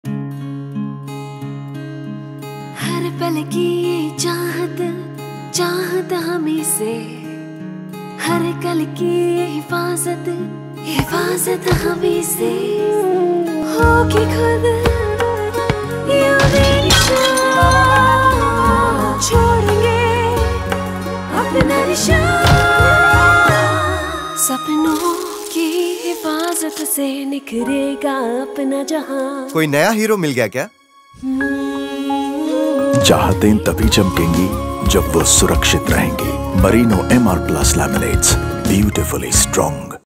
Freedom To Follow Your Dreams with Merino Laminates | Happy Independence Day
Freedom begins at home. Our homes are where we grow, learn and dream. They are the places where we can express ourselves and pursue our passions. With Merino’s scratch, impact, and stain-resistant surfacing solutions our homes remain resilient and durable, just like our spirit. Our homes are where we realize our true potential and make a difference in our own ways.

This Independence Day, let’s celebrate the freedom to chase our dreams.